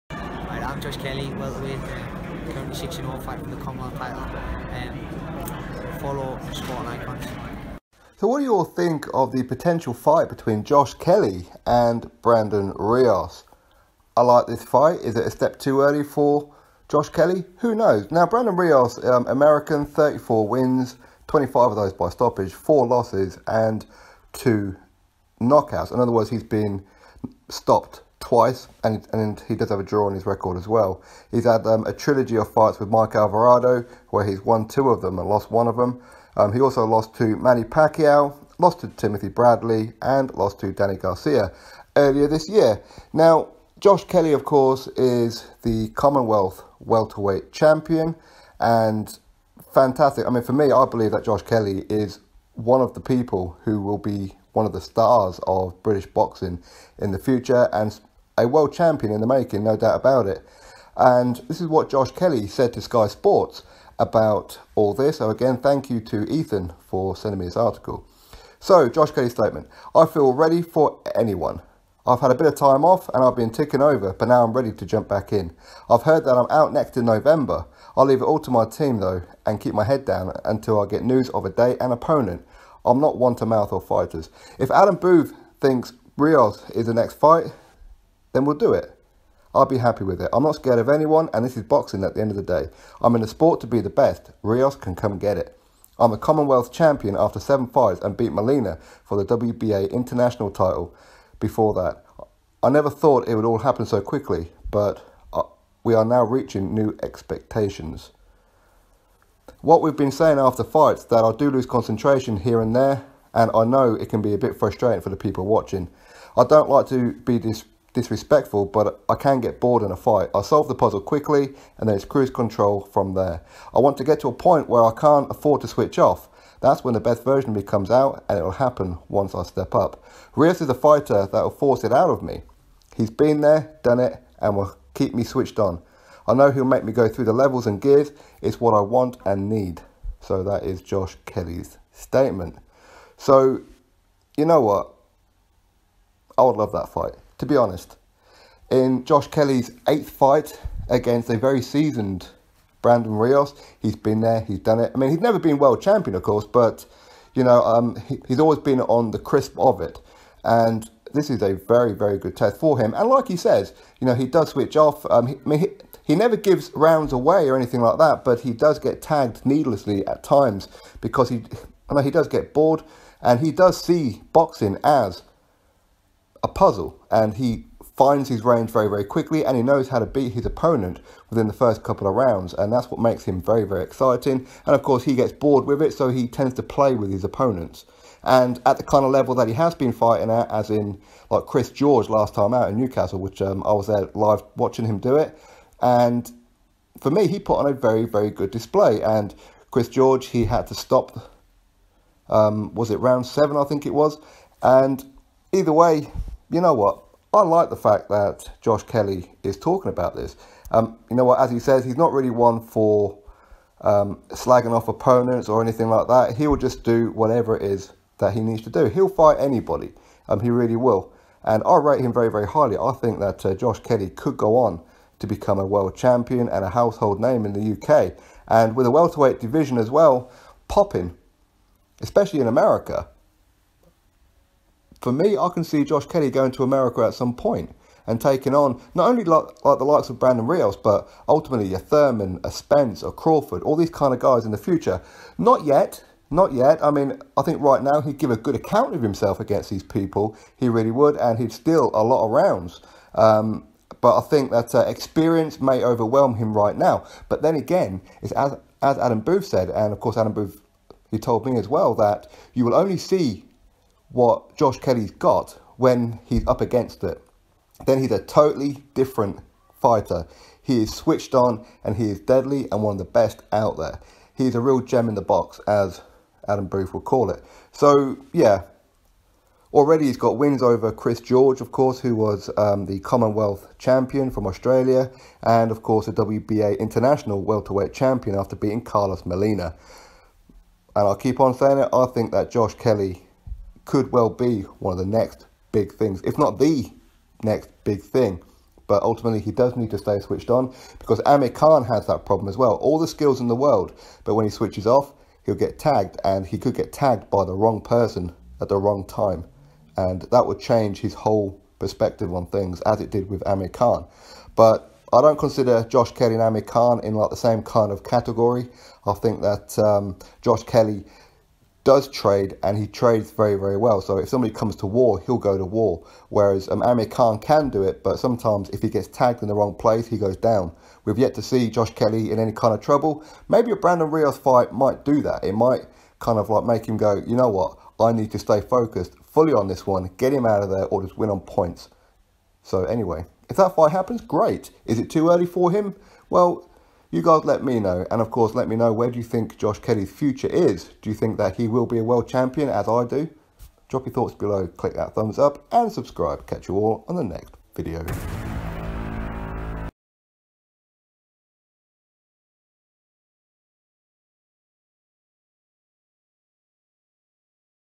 JOSH KELLY SAYS BRANDON RIOS IS A FIGHT HES WILLING TO TAKE NEXT!!
PBK Joshua Kelly has told Sky Sports that if his trainer Adam Booth says he should face Brandon Rios next then bring it on.
Josh Kelly is the Commonwealth and WBA International Welterweight Champion after defeating Kris George last time out in Newcastle Upon Tyne.
With the Welterweight division stacked with elite boxers such as Errol Spence Jr, Amir Khan, Terence Crawford, Manny Pacquiao, Keith Thurman and others, Josh could soon be in the mix for a top fight with one or a couple of these names.
For now, how do you see Josh Kelly vs Brandon Rios playing out?